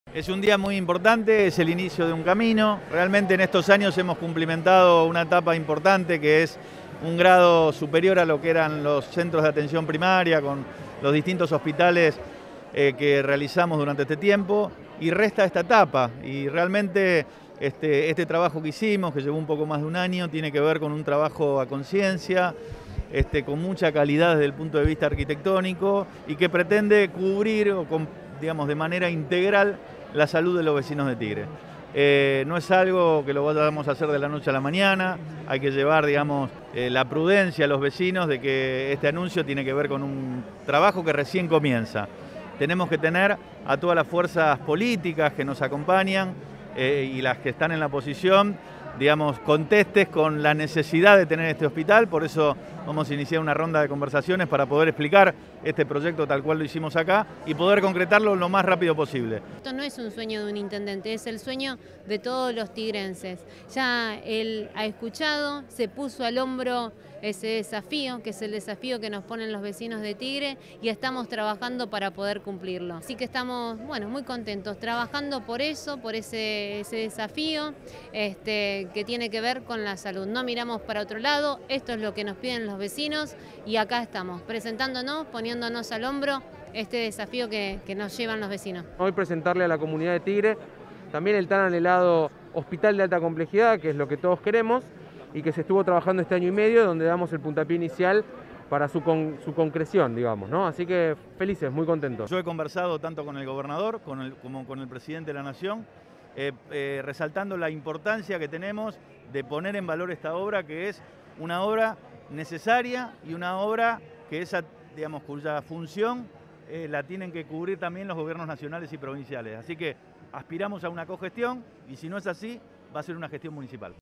Presentación del proyecto del nuevo Hospital Municipal de Alta Complejidad de Tigre
El intendente, acompañado de la concejala Gisela Zamora, comunicó a vecinos y vecinas de todo el distrito la propuesta para la construcción del edificio de salud, que contará con 142 camas de internación en una primera etapa y 464 camas una vez culminada la obra. “Vamos a cumplir con este sueño para cubrir la salud integral de toda la población”, aseguró el jefe comunal.